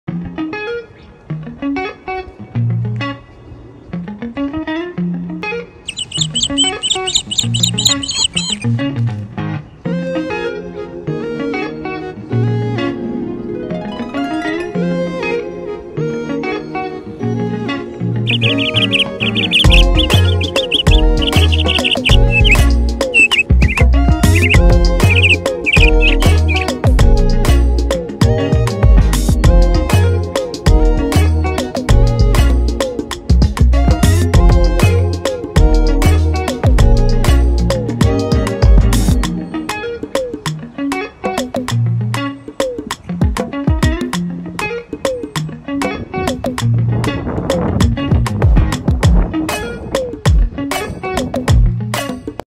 Morning walk in one of the beautiful parks in Manchester United Kingdom
Woke up early in the morning in Manchester and wanted to breath fresh air the weather was amazing outside I decided to take a short walk in one of the park near to the airport of Manchester 😊